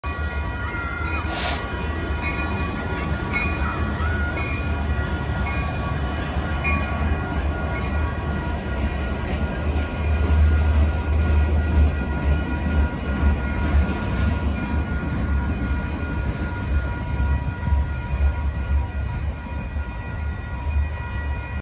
Union Pacific with only 3 cars and a caboose!!
I did some research, and this caboose used to belong to the Missouri Pacific Railroad. The UP bought them in 1997, and I have no idea what it was doing in a Chicago suburb.